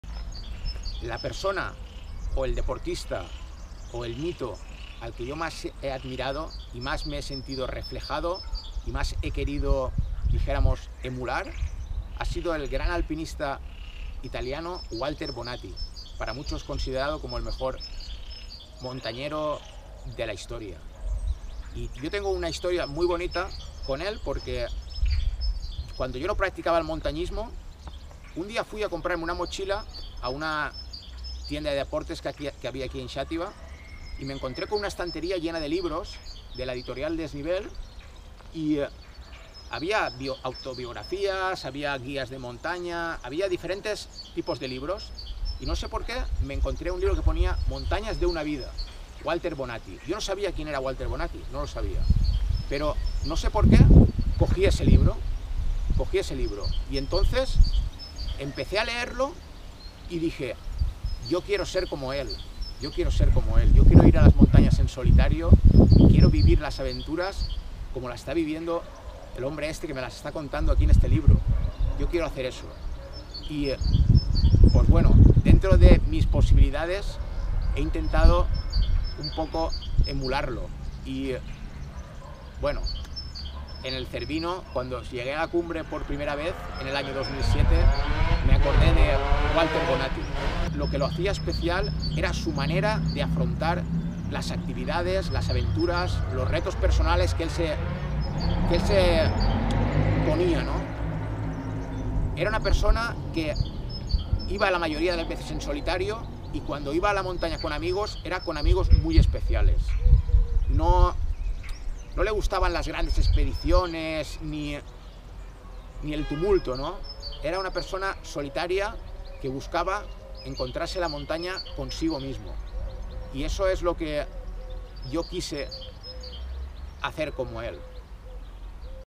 LA PREGUNTA #7: ¿Cuál a sido el mito al que más has admirado y por qué?
Entrevista a Javier García Mascarell en el jardín del Palasiet de Xàtiva el 24 de abril 2022